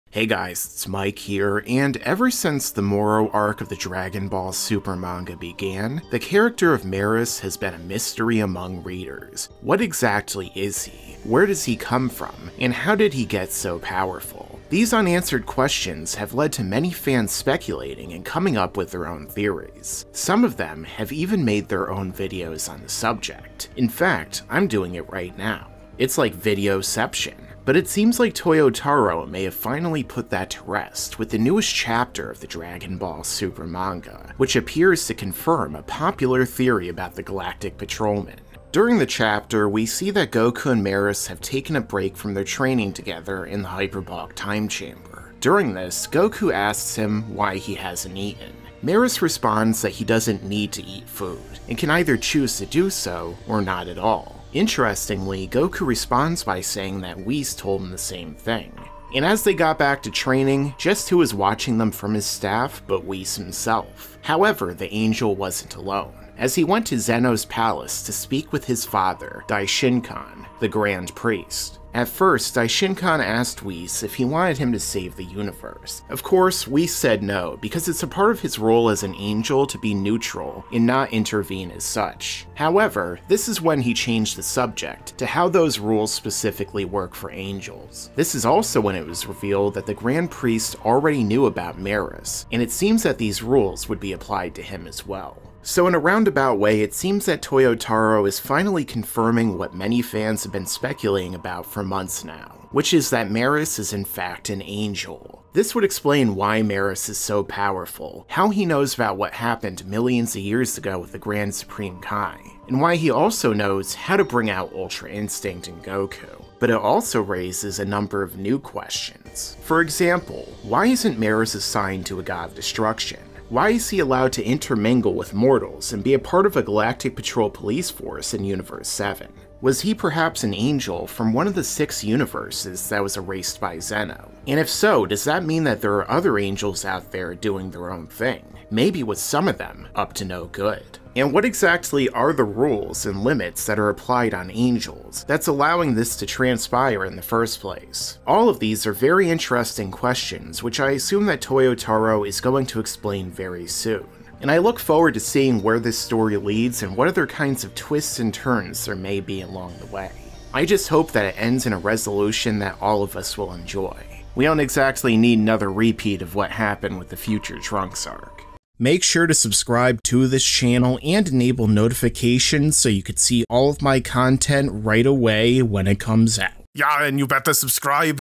The TRUTH About MERUS Finally REVEALED
Is Merus an ANGEL? Is Merus secretly EVIL? What does Whis want to talk about with Daishinkan the Grand Priest? Laughingstock Media discusses the reveal made in Dragon Ball Super chapter 54, featuring the Dragon Ball Super anime and manga!

RECOMMENDED CHANNELS